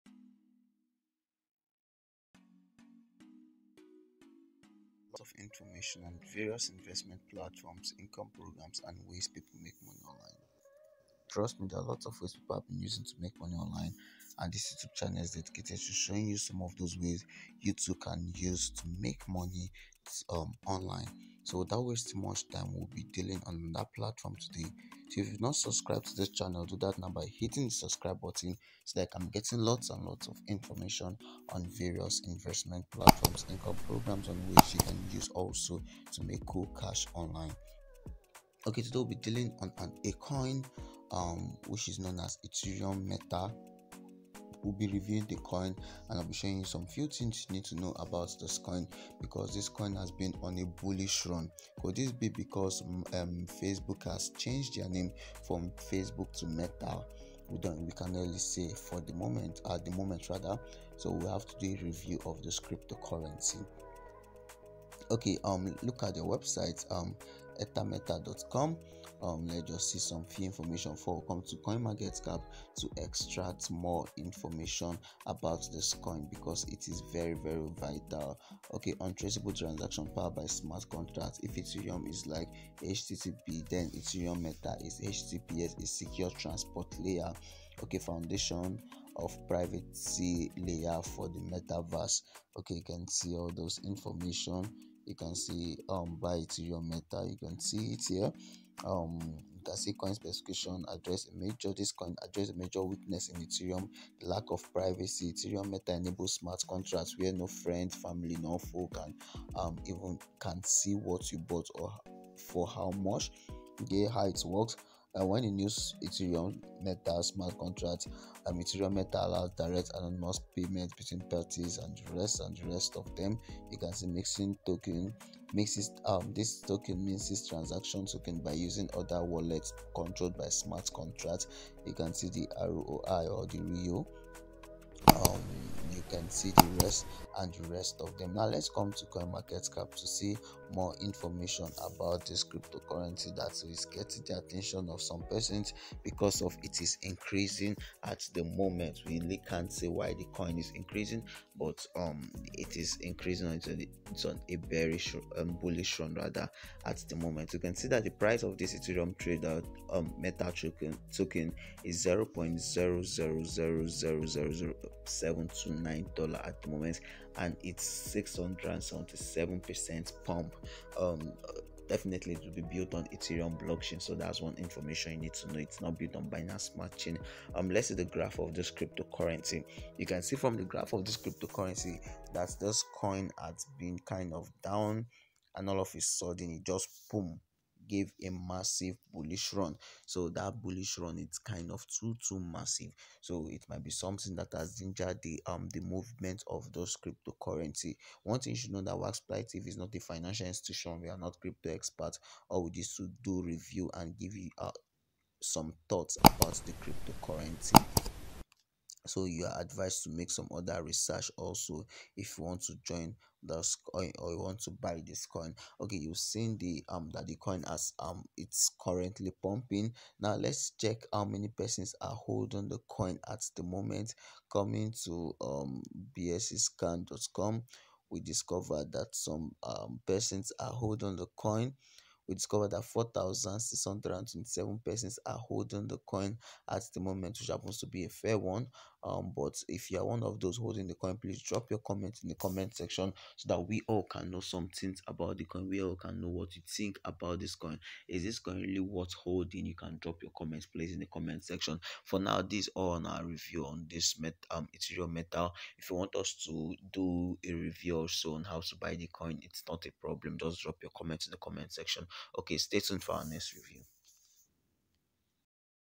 Ethereum Meta Token Review || Is 1000x Achievable? || Price, Prediction and Updates of Ethereum Meta
There are lots of coins coming online and one has to know about them. Ethereum Meta Token Review will give you information you need.

daily to get lots of information on ways you can make money online.